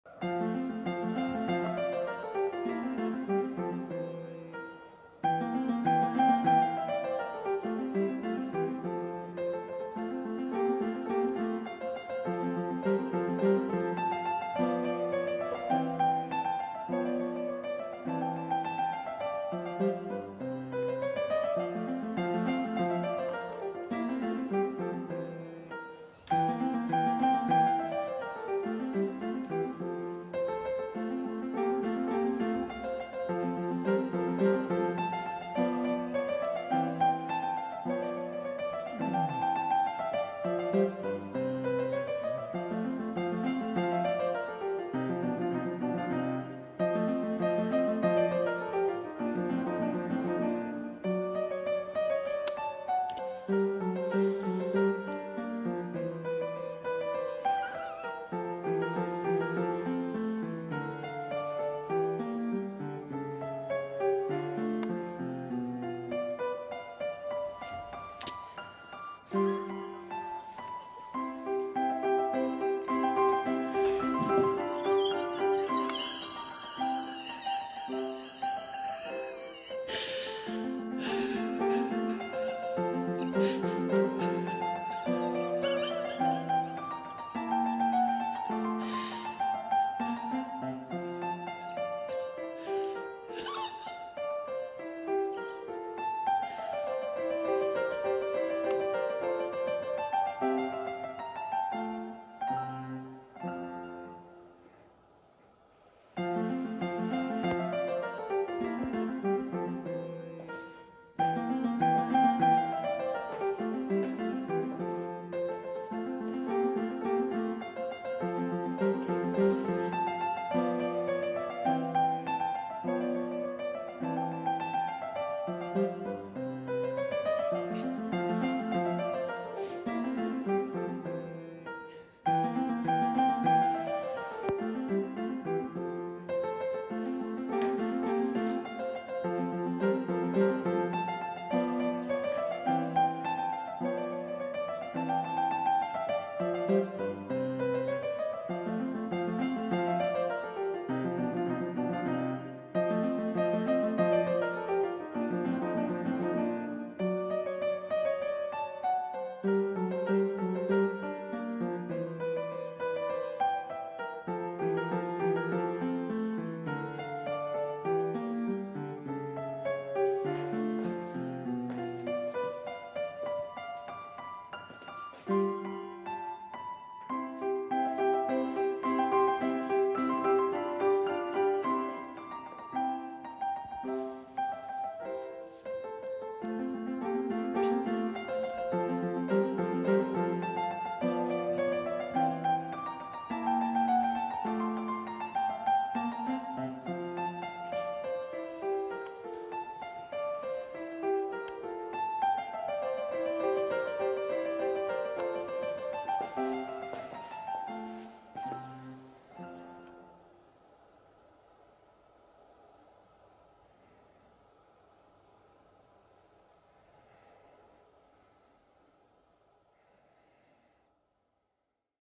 Weird Cat Making A Cocoon Bed Out Of Curtains
My weird cat Indy is making a cocoon bed out of curtains in this video.. This is how he's been making his own bed and tucking himself in recently !!